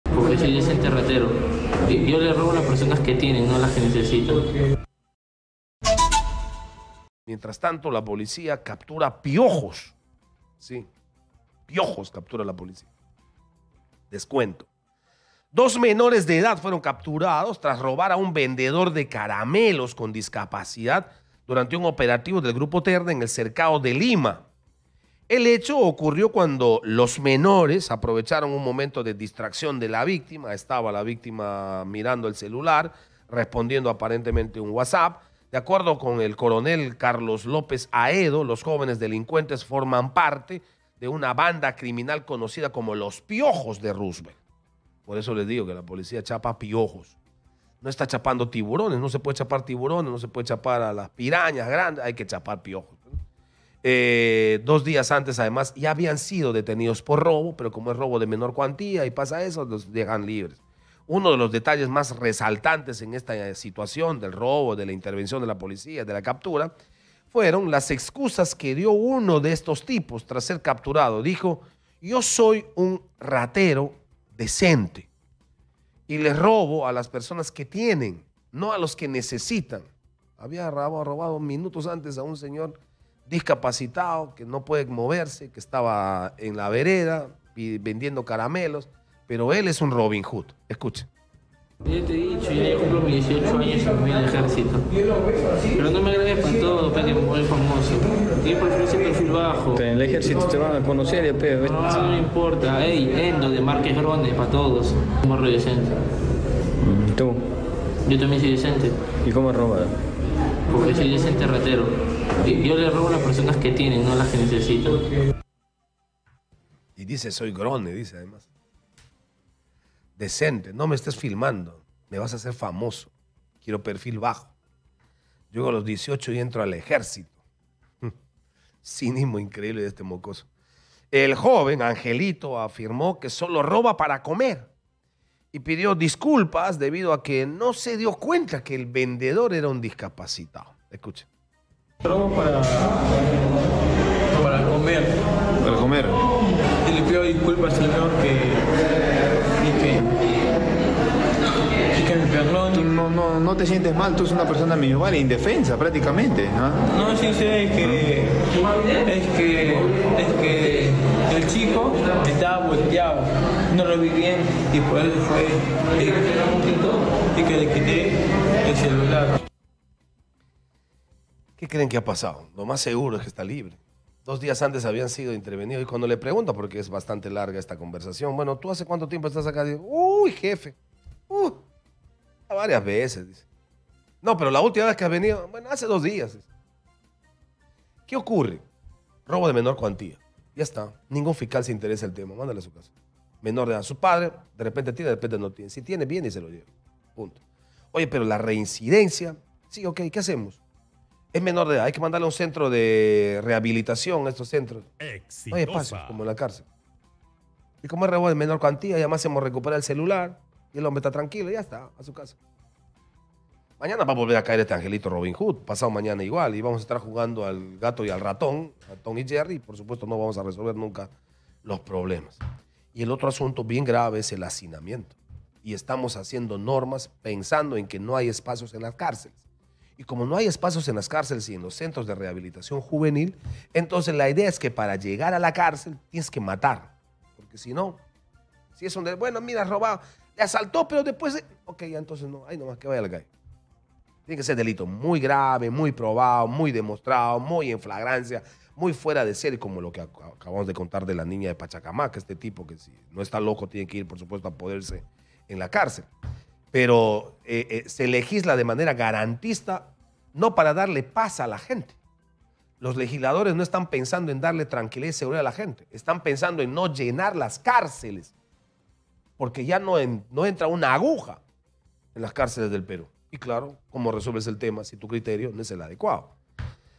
🔴🔵"Soy un ratero decente": Menor se confiesa tras hurtar celular a vendedor discapacitado
Tras un operativo, dos menores de edad fueron capturados por robar a un vendedor de caramelos con discapacidad. Uno de ellos confesó su fechoría y dijo: "Yo le robo a las personas que tienen, no a las que necesitan".

Exitosa Perú
"Exitosa: La voz que integra al Perú"

Corporación Universal